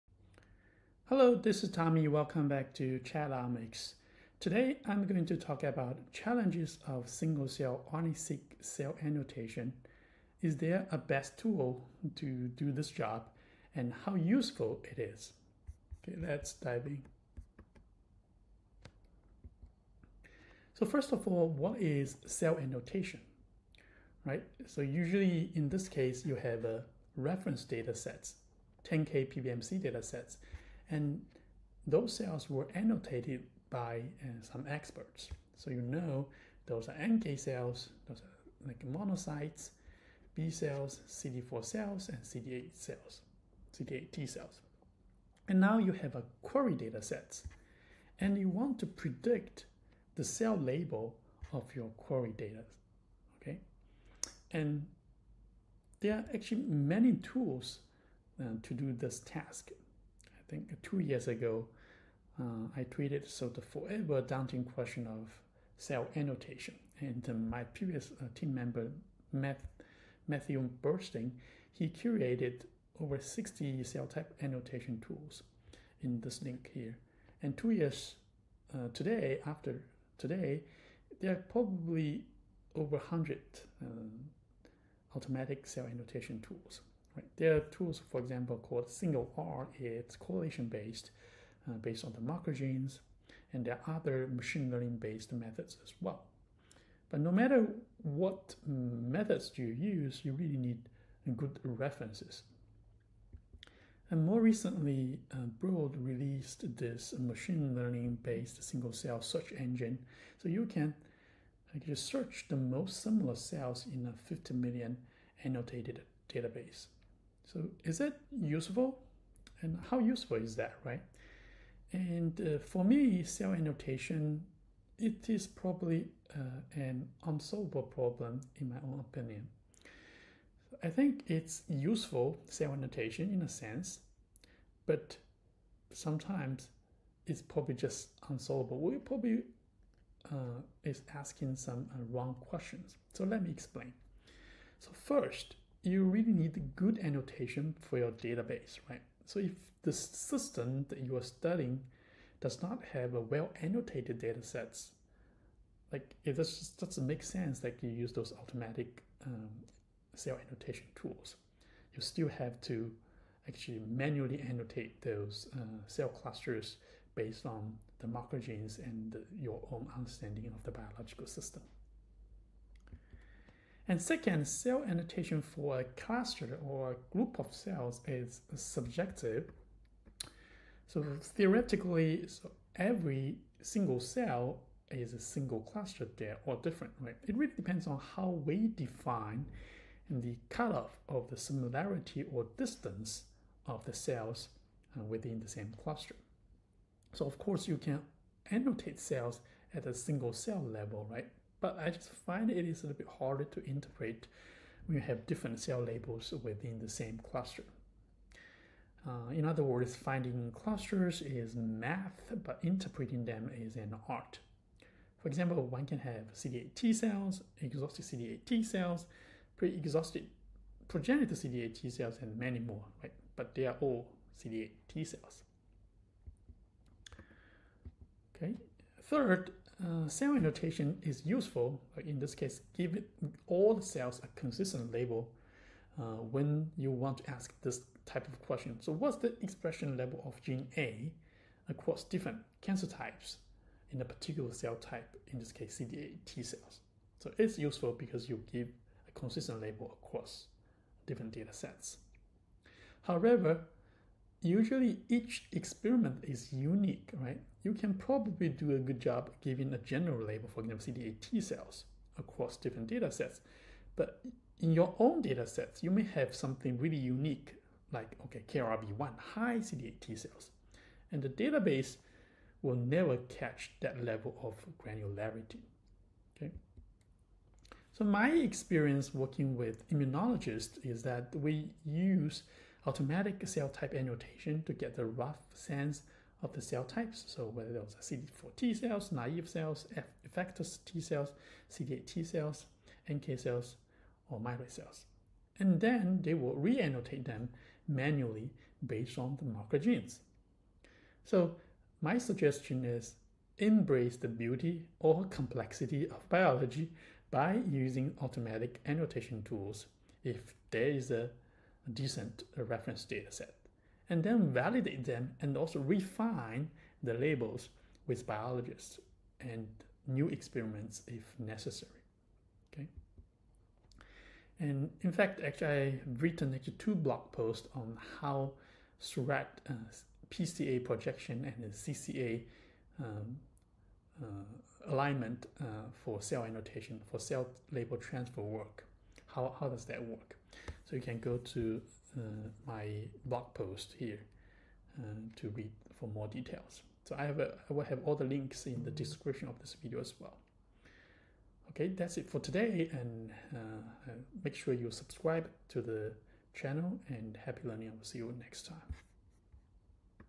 SINGLE CELL RNAseq cell annotation Mystery Revealed!
Single cell annotation is a challenging problem. In this video. I dive into my experience of automatic cell annotation tools and share tips when you deal with your own data.